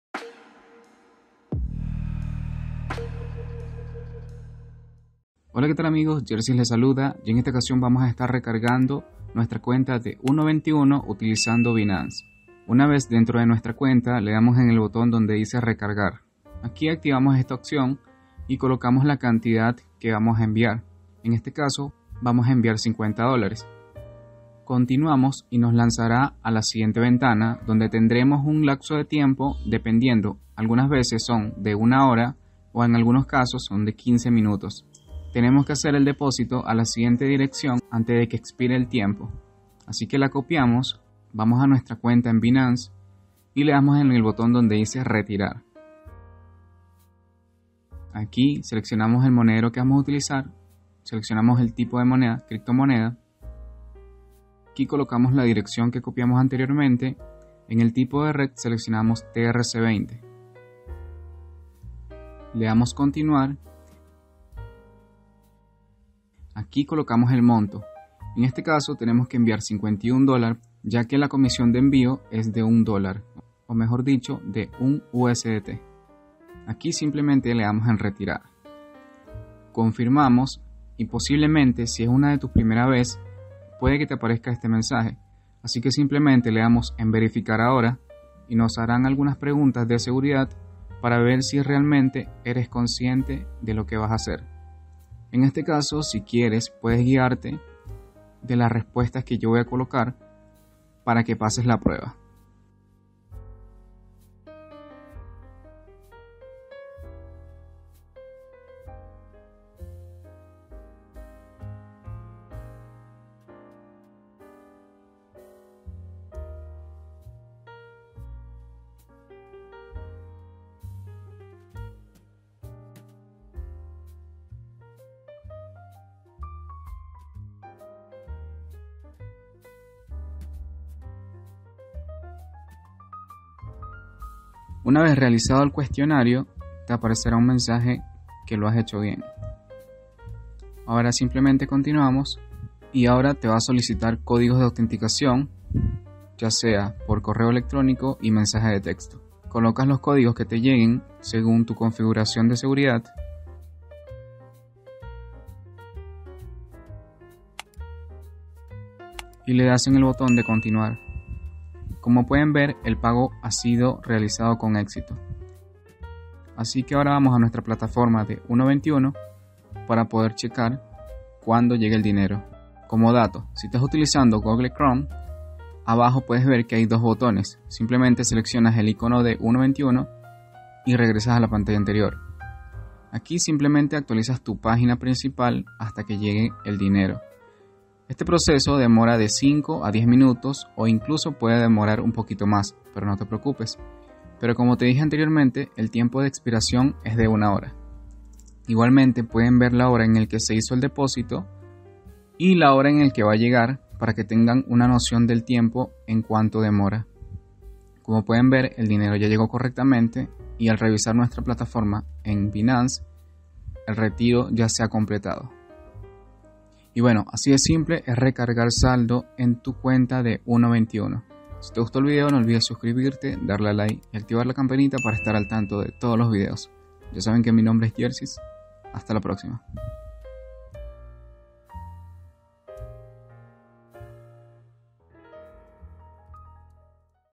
🌐 U91🏅 - ⚽Como RECARGAR 💰 saldo a tu cuenta en U91 utilizando BINANCE 🔰 (BIEN EXPLICADO) 2022 💎
Hola amigos, Jercyz 🦊 les saluda, en este video estaremos explicando 🌐 U91🏅 - ⚽Como RECARGAR 💰 saldo a tu cuenta en U91 utilizando BINANCE 🔰 (BIEN EXPLICADO) 2022 💎